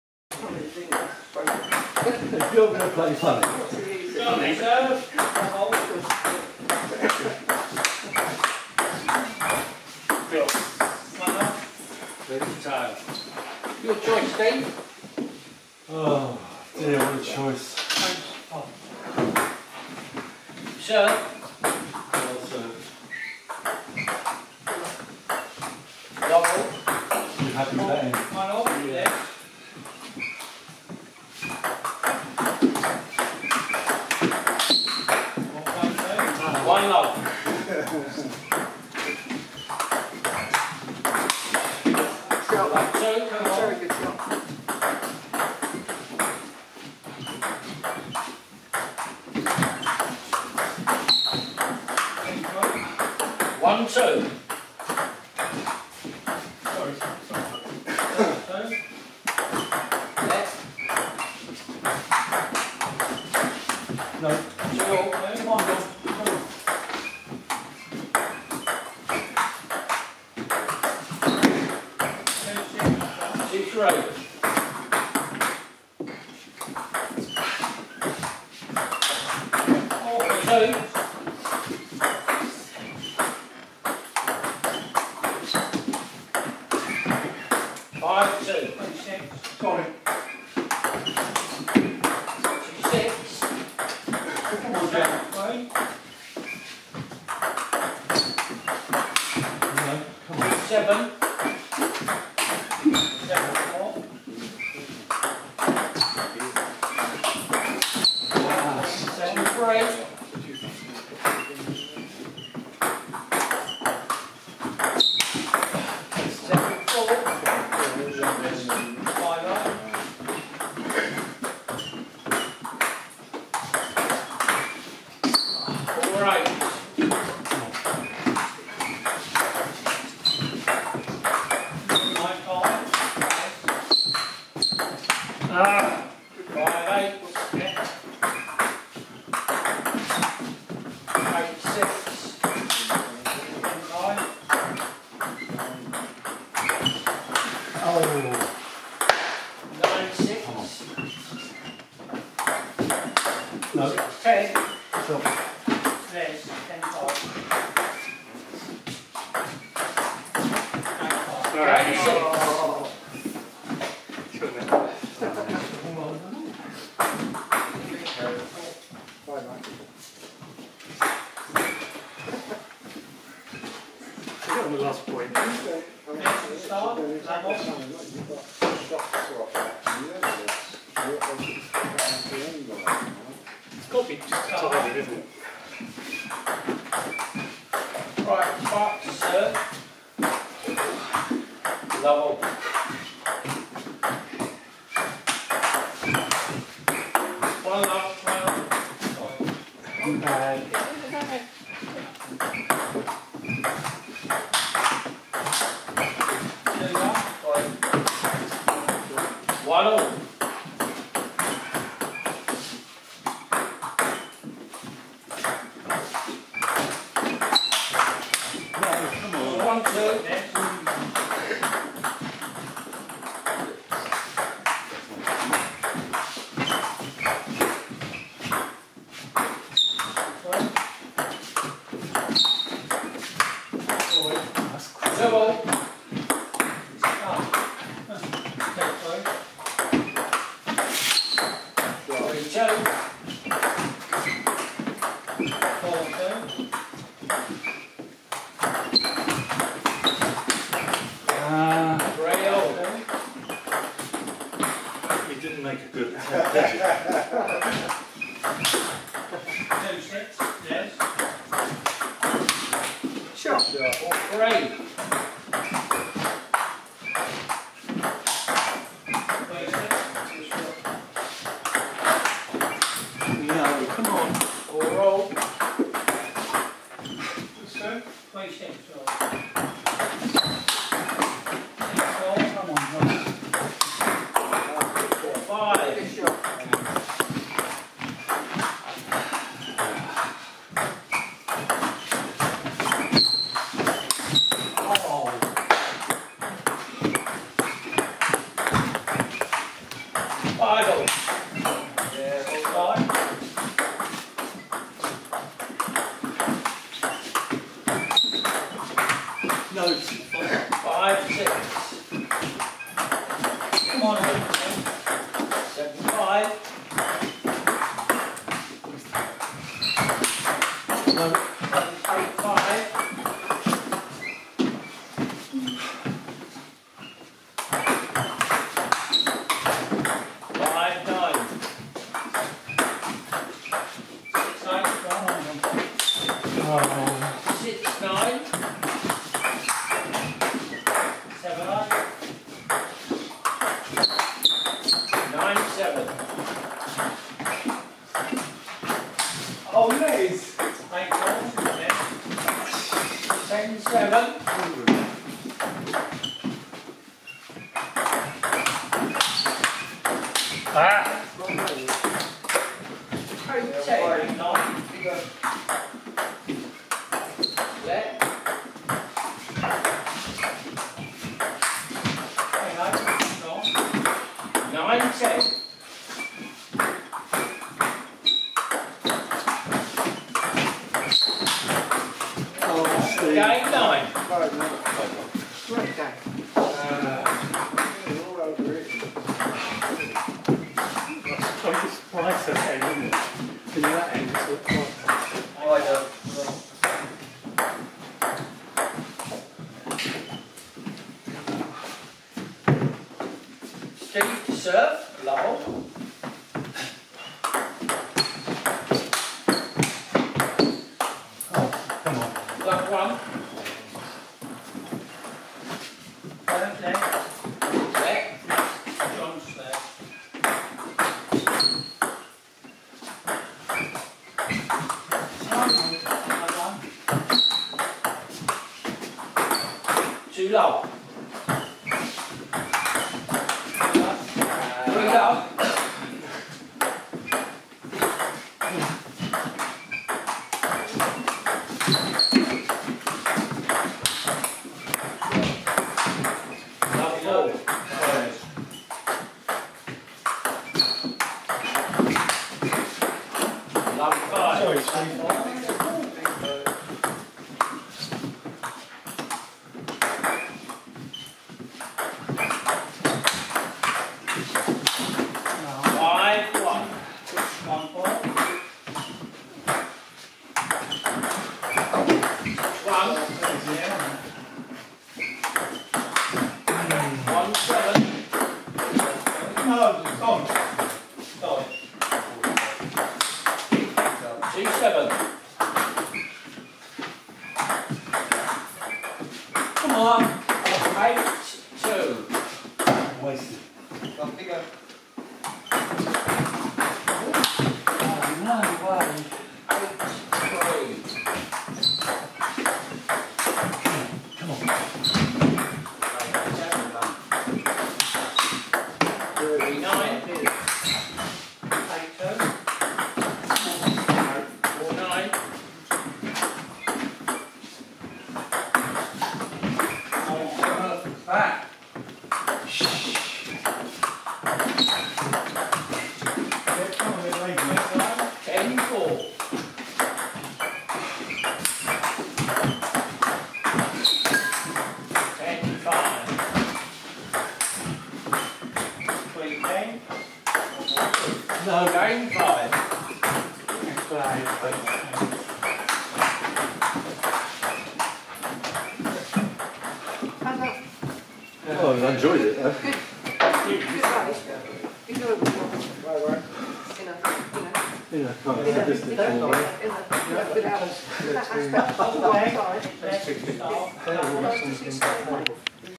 STEPHEN MERCER VERSUS MARK REDFORD 290923
Table Tennis Bedford League Match Div 2.